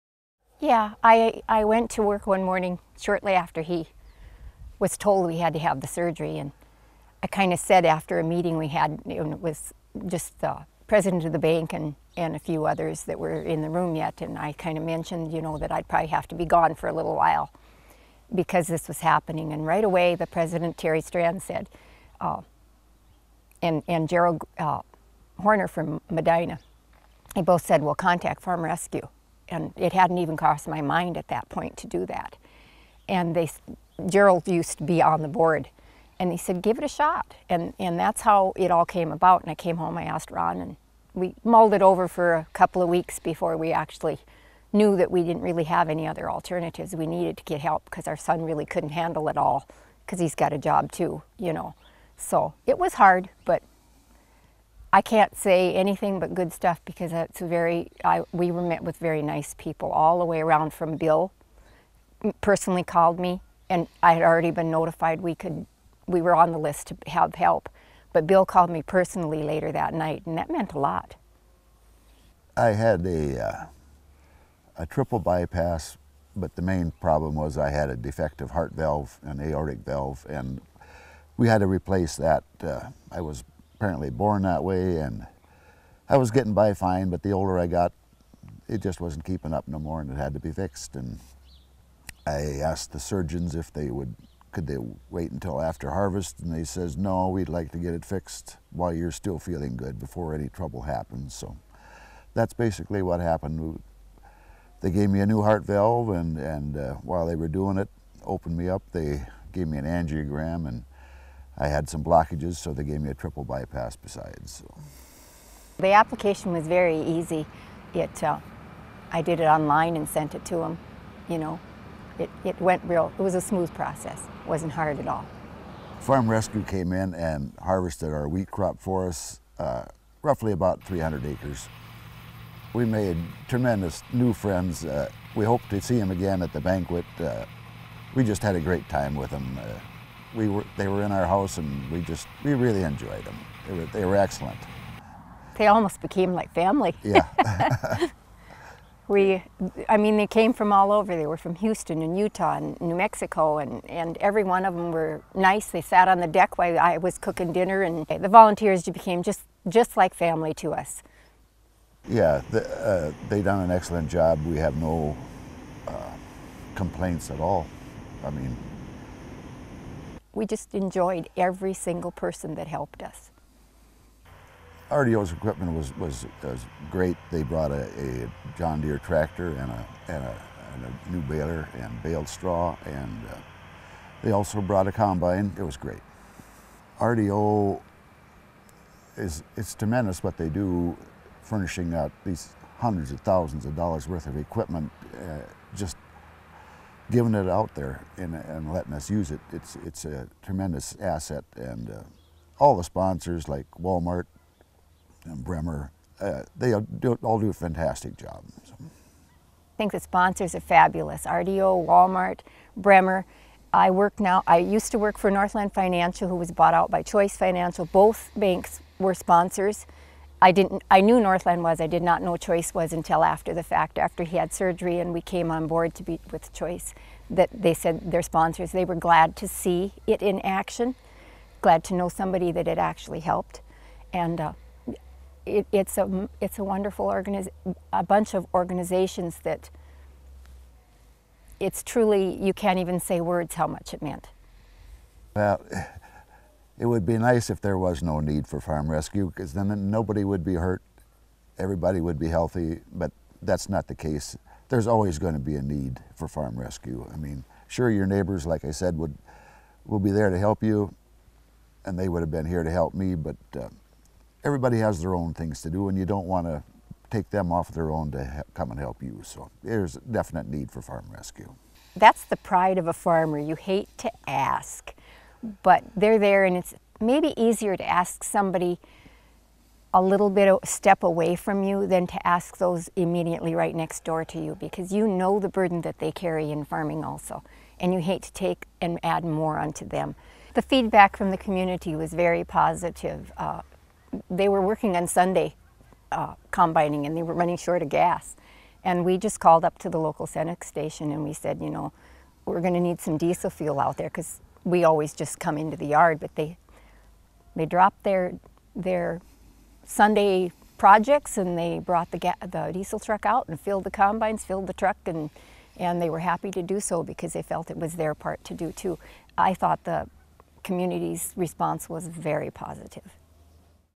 FARM RESCUE - 2015 - Pettibone, North Dakota
Ron & Robin Rewald speak of assistance received from Farm Rescue due to Triple-bypass heart surgery.